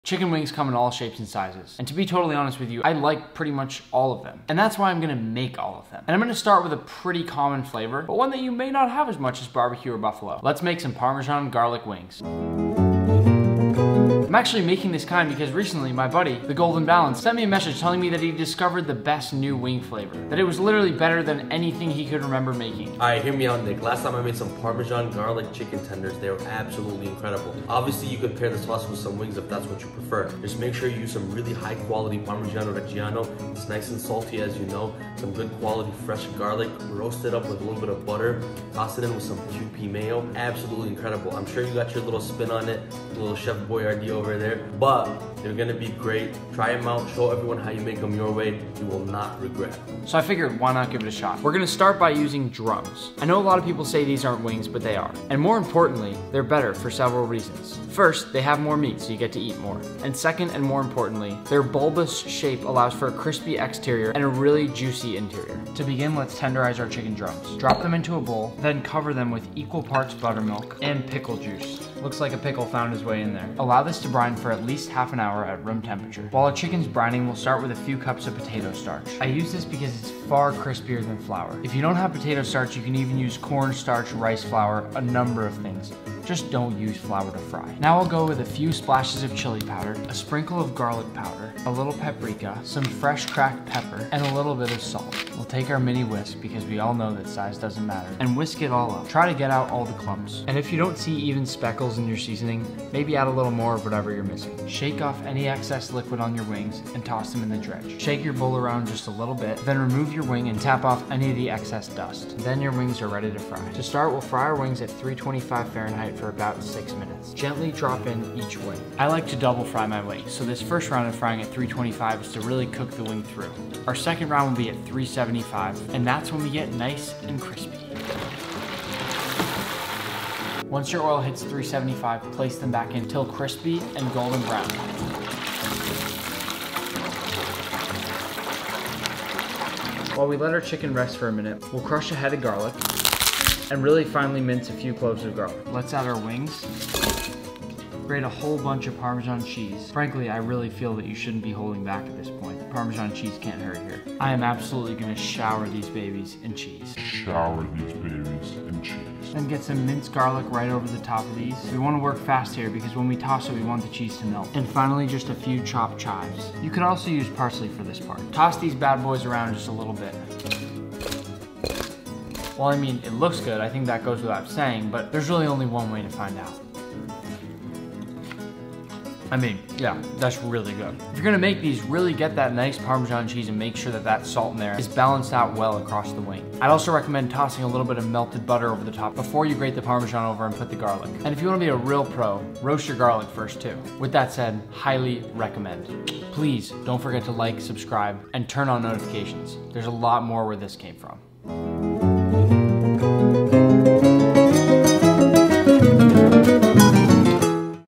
Parmesan Garlic Wings
Try making these wings! You'll love them.

Ingredients:
6 chicken drums
2 cups buttermilk
2 cups pickle juice
3 cups potato starch
pinch of chili powder
pinch of garlic powder
pinch of paprika
Frying oil
4 tbsp butter
4 cloves garlic
2 cups Parmesan cheese
1/3 cup chopped chives

1. Marinate chicken in buttermilk and pickle juice for 30 minutes.
2. Combine potato starch, chili powder, garlic powder, paprika, salt and pepper in medium sized bowl.
3. Dredge wings in starch mixture. Shake off excess.
4. Fry for about 6-8 minutes at 325 F. Set aside.
5. Fry another few minutes at 375 F until golden brown and crispy.
6. Optionally, melt butter in a small saucepan.
7. Add garlic, parmesan, and chives and coat this (with butter if desired) over wings. Toss and enjoy!

follow all of these for a wing